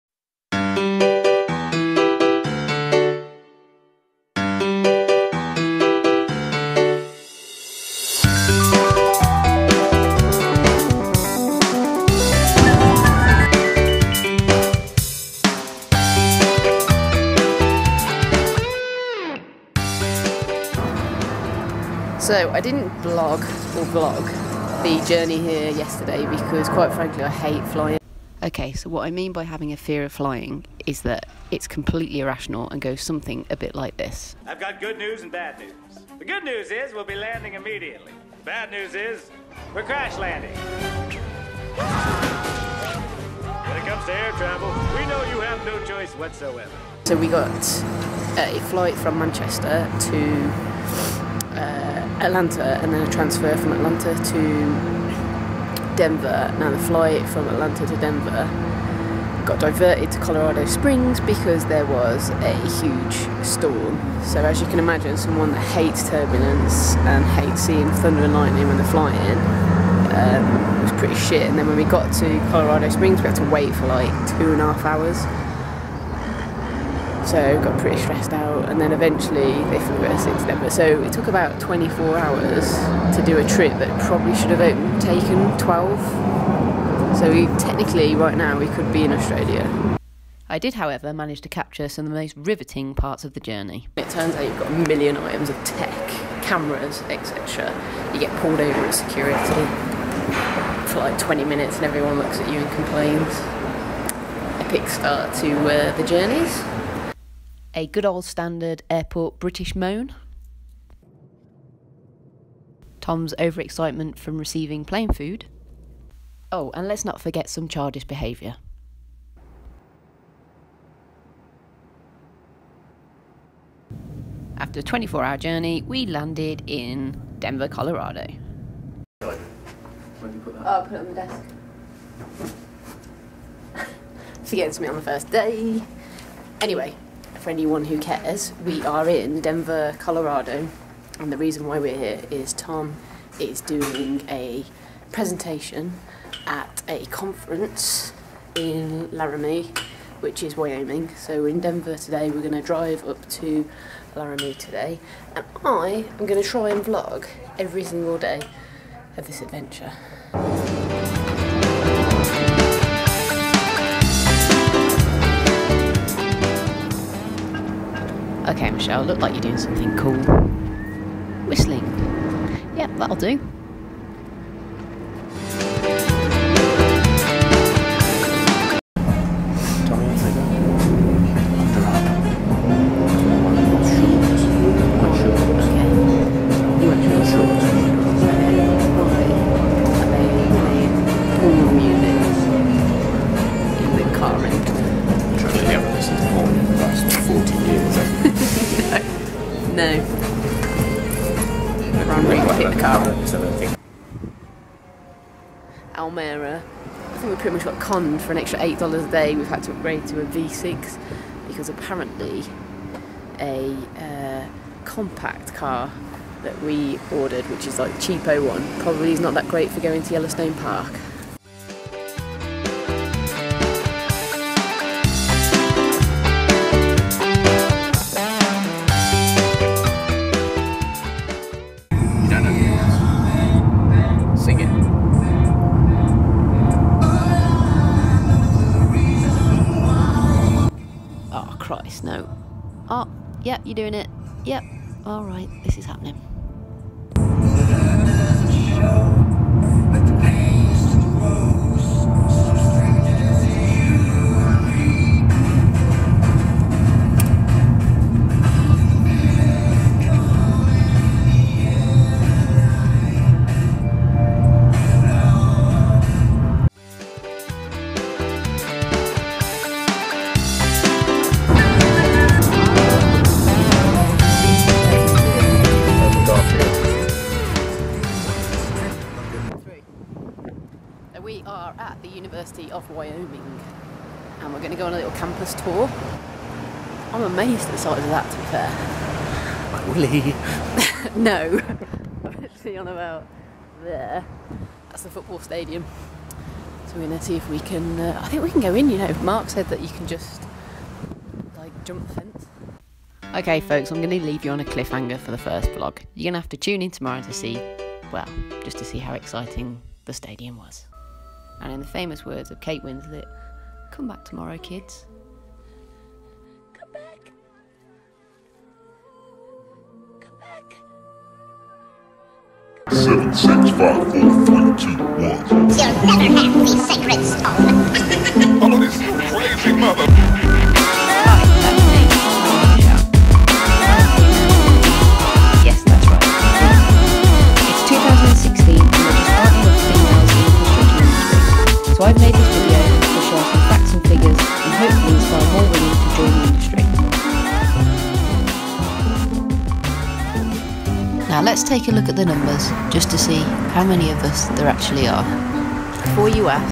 Vlog 1- Crazy/Scary Flight to Denver
The first of my travel daily Vlogs for our US Road trip. If you are wanting an informative travel Vlog. please go elsewhere. If you would like to see a stupid British women get up to some crazy shit, then welcome!